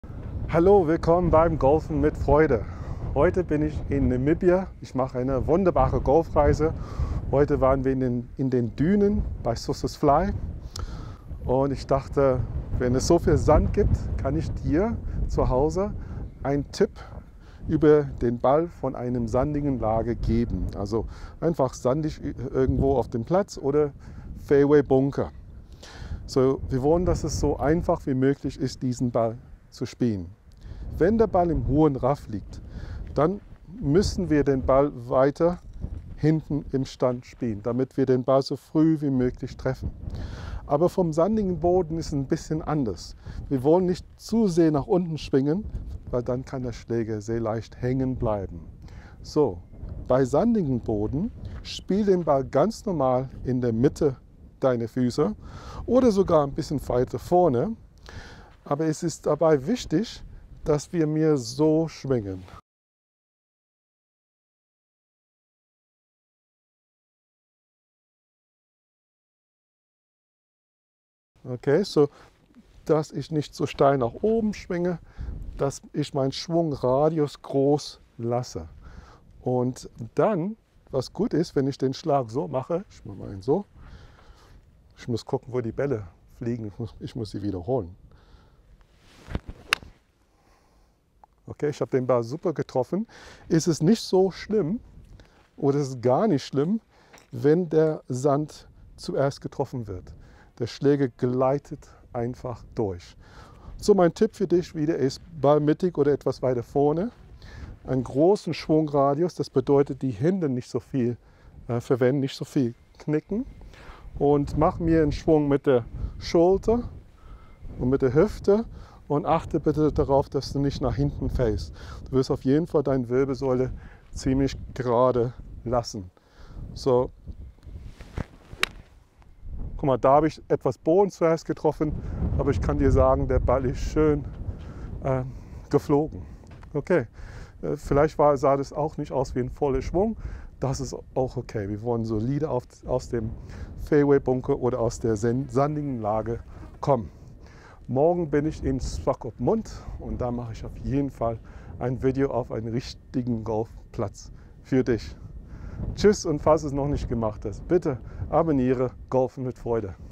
Die einfachste Technik für weichen Boden (nass oder sandig)
Wenn dein Ball auf Sand oder weichem Boden liegt, versuche, den Ball wie in diesem Video gezeigt zu spielen - weiter vorne im Stand und mit weniger Handgelenk.